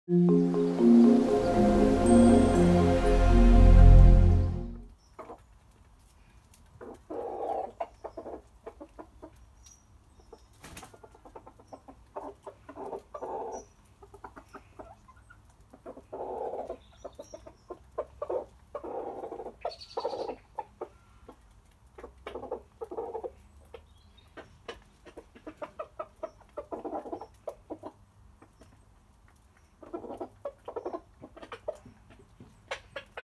Bantam Chickens | Eating
Today's video was a short video of Reji (a Maran and Pekin breed) sharing his food with the ladies. I thought it was cute, and that you guys might enjoy it!